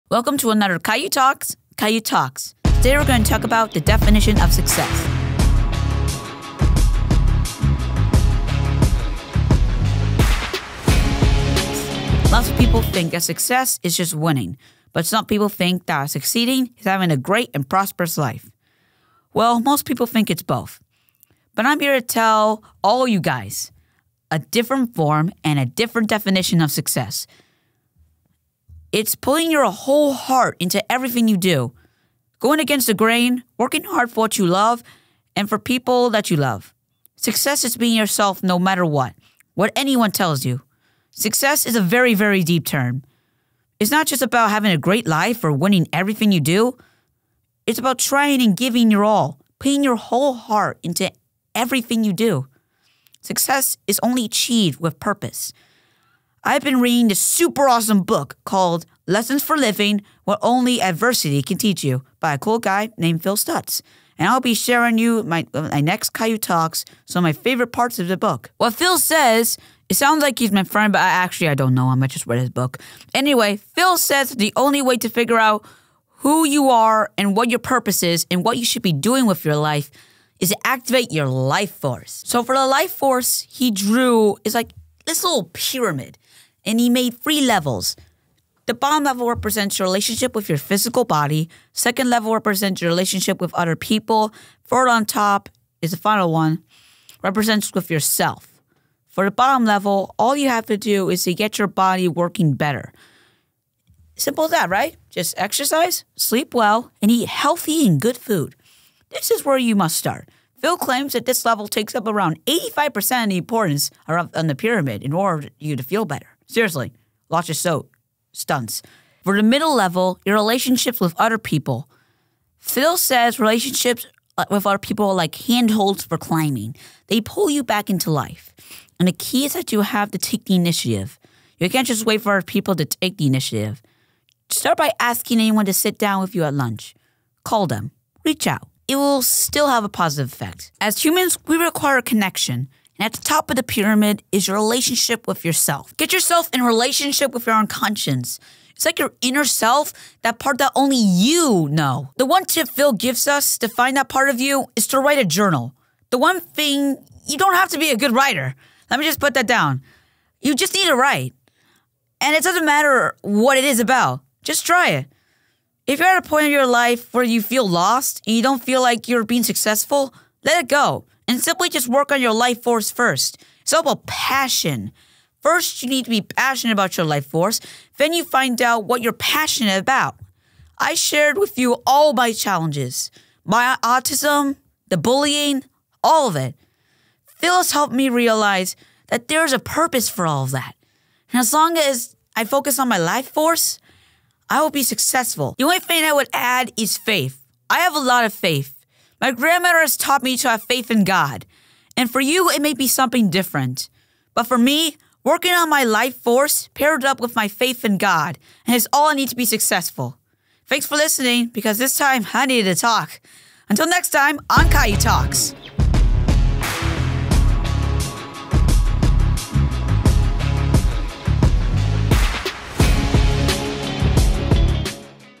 The Definition Of Success
Caio Ninja dives into the definition of success.

Timestamps:
00:00:00 - The Definition of Success
00:00:15 - A Different Definition of Success
00:00:27 - Success and Authenticity
00:00:39 - The Meaning of Success
00:00:50 - Success with Purpose
00:01:04 - My Favorite Parts of Phil's Book
00:01:13 - Activating Your Life Force
00:01:25 - Levels of the Pyramid
00:01:35 - Getting Your Body Working Better
00:01:47 - The Importance of Good Food
00:01:59 - The Importance of Relationships with Others
00:02:10 - Taking the Initiative
00:02:21 - Building a Relationship with Yourself
00:02:32 - Finding Yourself Through Journal Writing
00:02:43 - Working on Your Life Force
00:02:53 - Discovering Your Passion
00:03:06 - Finding Purpose and Faith in Life
00:03:19 - Faith in God and Life Force
00:03:30 - Caio talks